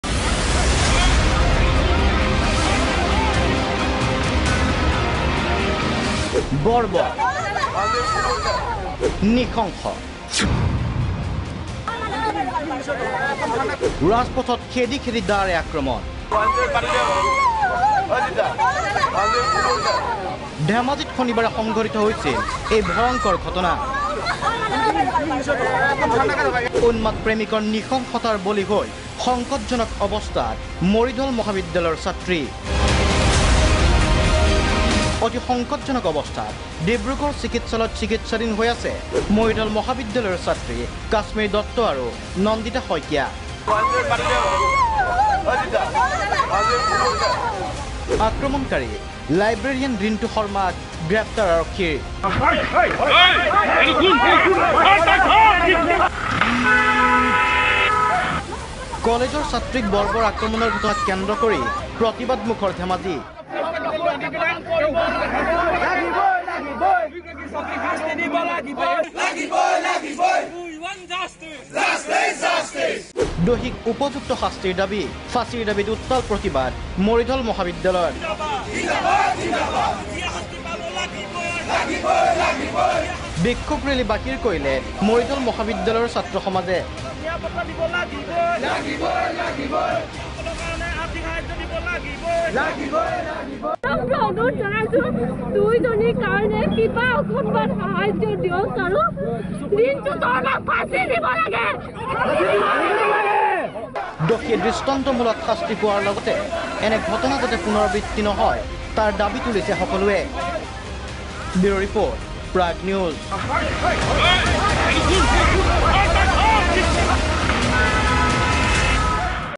Dhemaji: 2 College Girls Stabbed By Library Assistant !
Dhemaji: 2 College Girls Stabbed By Library Assistant ! Watch the full clip to know more about this story.